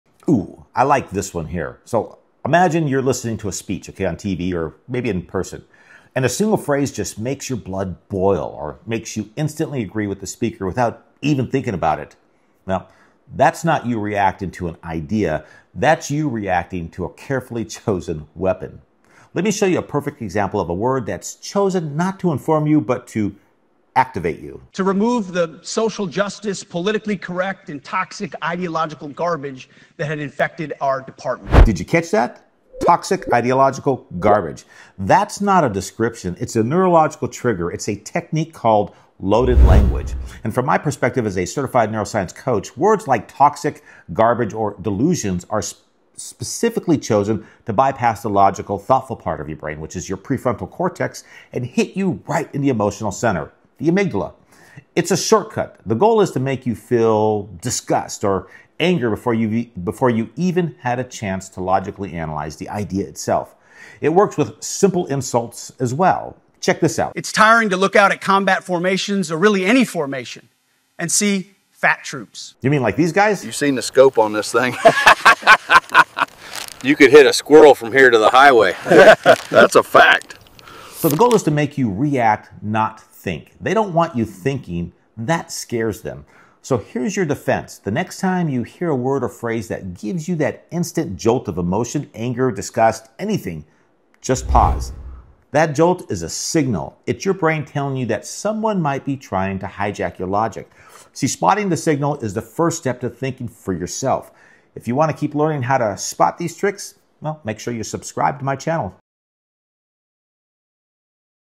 Your Brain on "Loaded Language"
Words can be weapons. Phrases like "toxic ideological garbage" aren't just descriptions; they're neurological triggers designed to make you feel instead of think. In this video, I break down the science of "loaded language," how it targets your brain's emotional center (the amygdala), and the simple pause you can use to take back control of your logic.